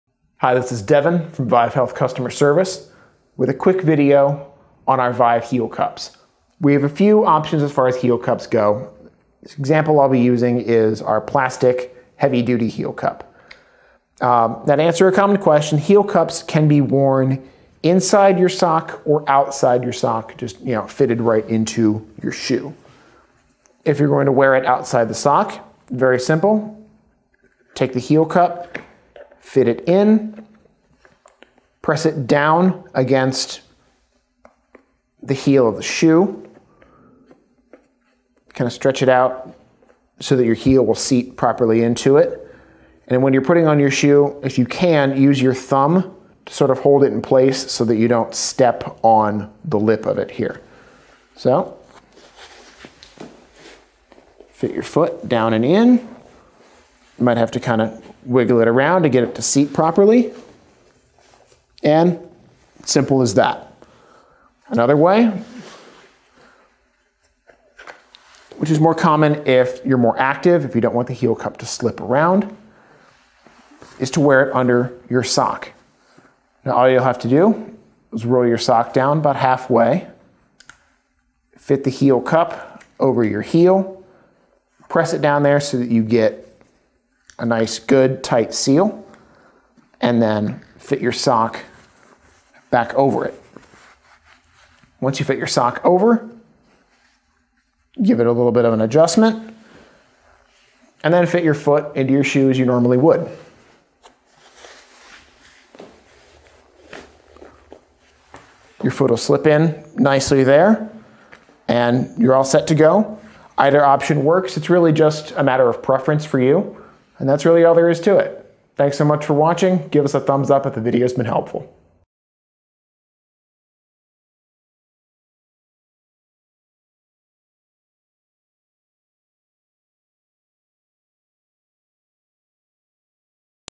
How to Use Heel Cups
Learn how to use the plastic Heel Cups by ViveSole. These heavy duty heel cups can be worn with or without socks to alleviate heel & foot pain. If you are going to wear them without socks, insert & position them at the heel of the shoe and slide your foot in.

If you are going to wear them with socks, place them underneath your heel and pull your sock over it, adjusting as necessary. These hard heel cups are great for relieving painful symptoms from plantar fasciitis, bone & heel spurs.

SKU #: INS1030S